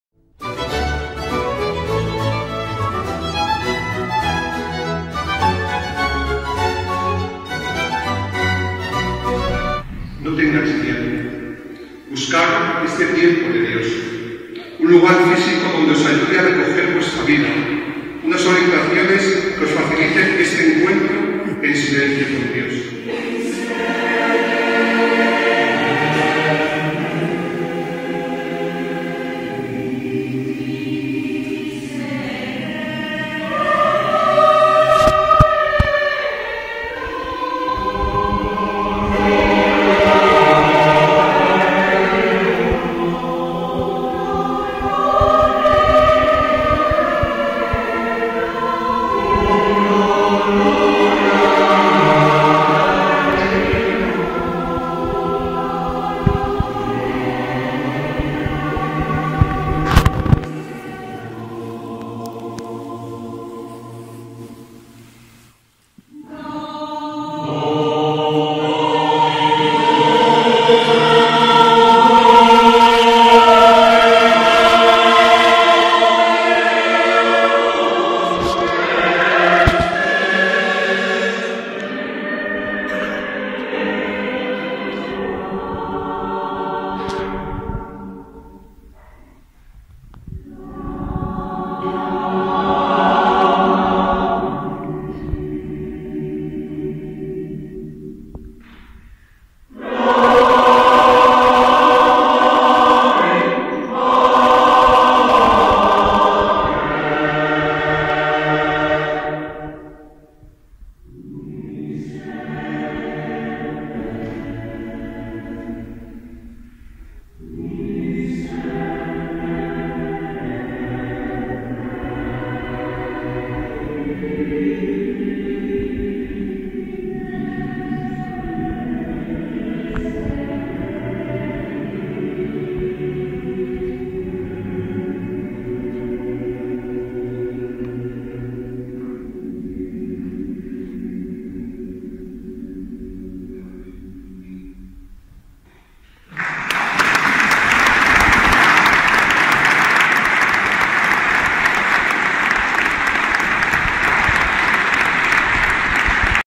Pregón Semana Santa Arévalo.
Desde la iglesia de Santo Domingo donde se venera a la patrona Virgen de las Angustias, don Jesús Sánchez Blázquez párroco de Arévalo; impartió lleno de fieles el pregón de la Semana Santa 2019.